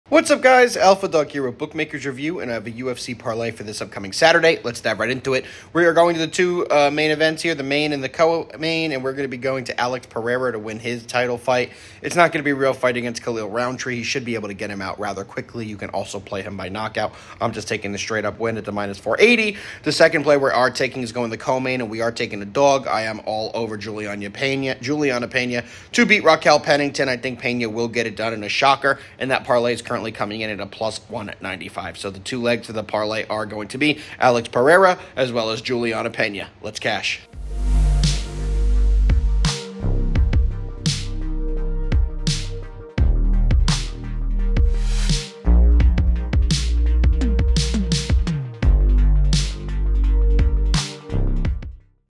UFC 307 | Free UFC Parlay and Analysis by Alpha Dog (Oct. 5th)
Prepare for an action-packed night at UFC 307 with Alpha Dog's UFC picks and free parlay! These UFC predictions promise thrilling opportunities for MMA betting enthusiasts with great value at +195, featuring two highly anticipated bouts:

Alex Pereira vs. Khalil Rountree Jr. 0:10
Raquel Pennington vs. Julianna Peña 0:22

Don’t forget to visit Bookmakers Review for the latest UFC odds!